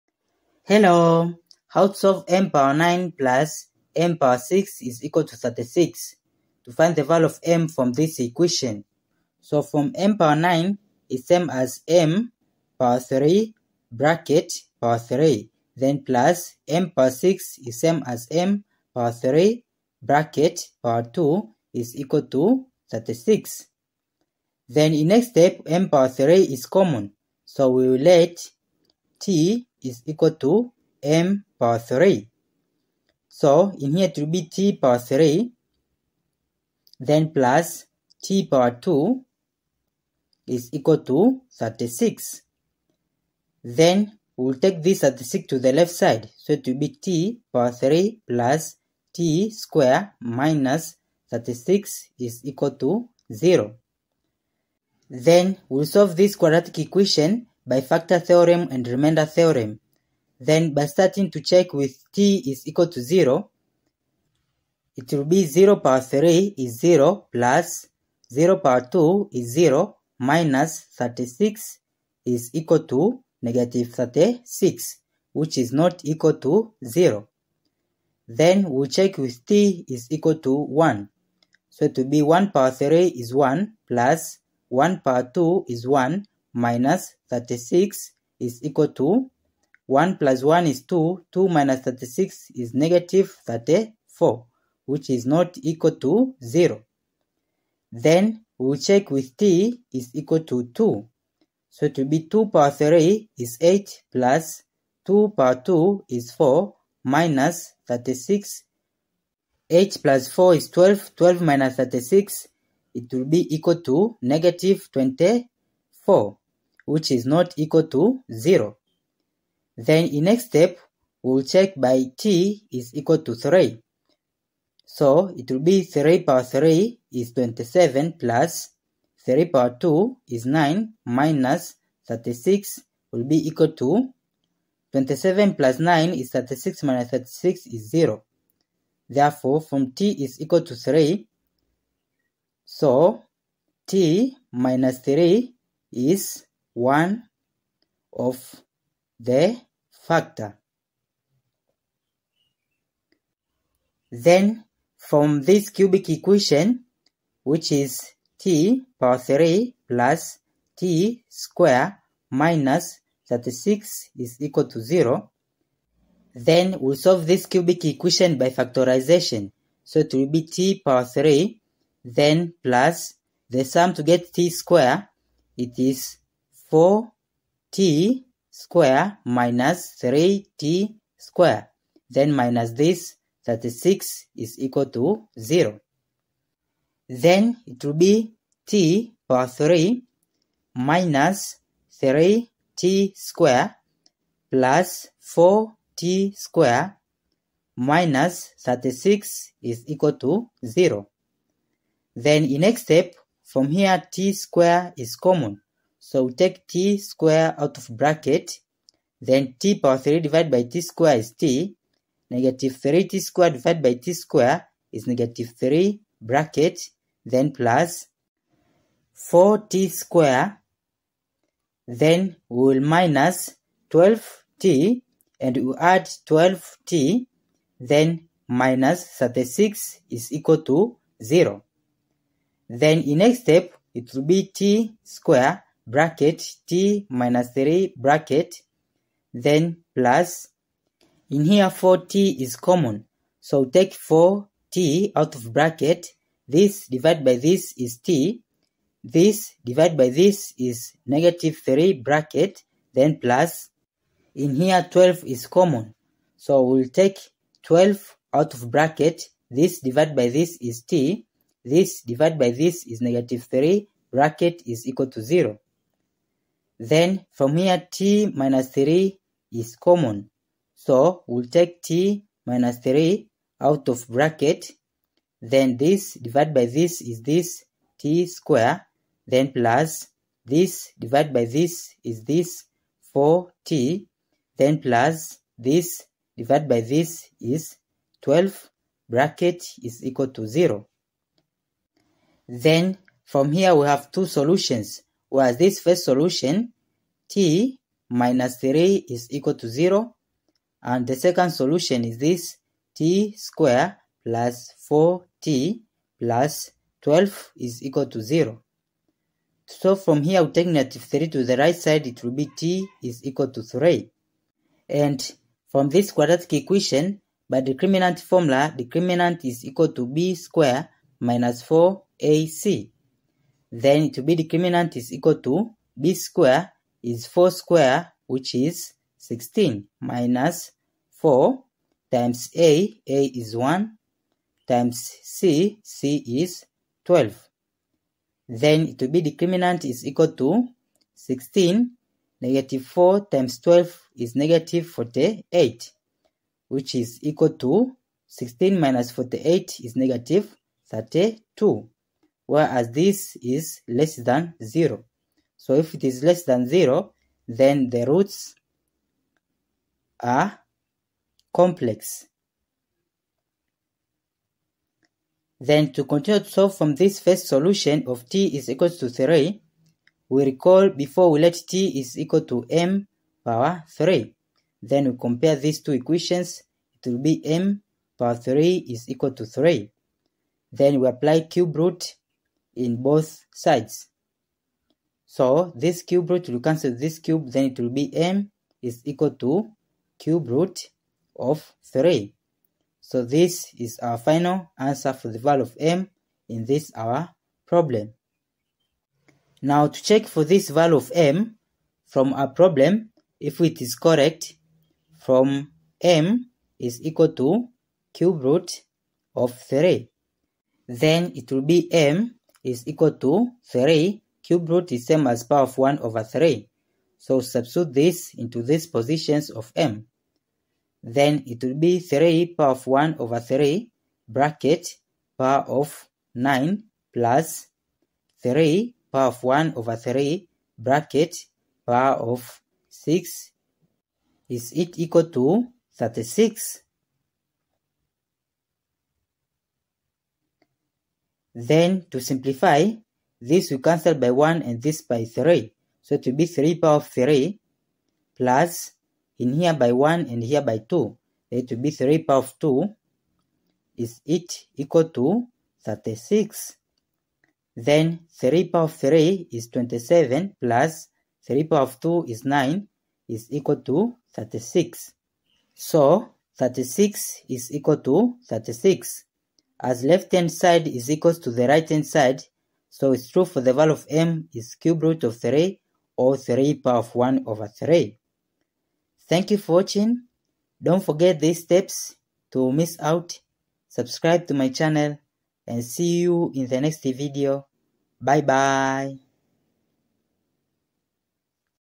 A Nice Algebra Problem
How to solve this algebra olympiad math problem